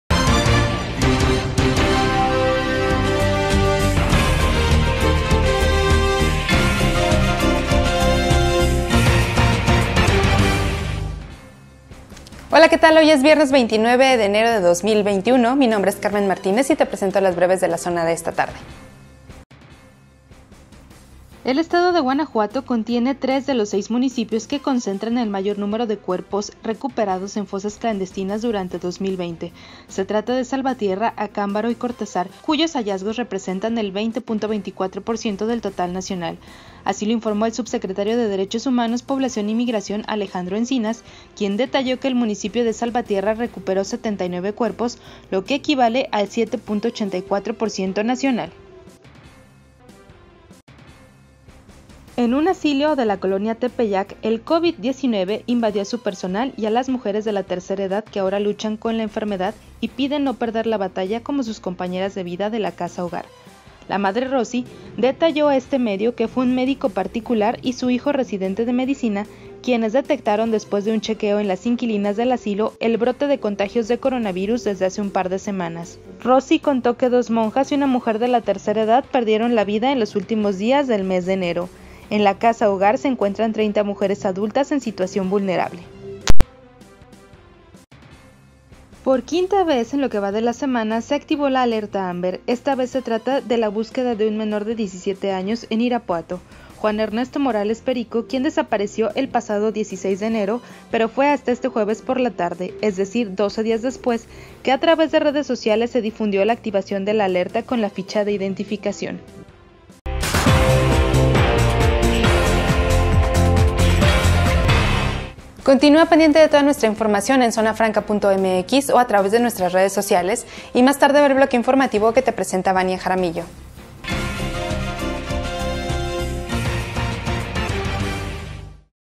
Irapuato activa quinta Alerta Amber en la semana | Breves de la Zona
- Con 4 búsquedas, Guanajuato concentra el 20% de cuerpos rescatados en fosas clandestinas.
- Ante brote de Covid-19 en asilo, requieren apoyo.
- Irapuato activa quinta AlertaAmber en la semana; buscan a menor desaparecido.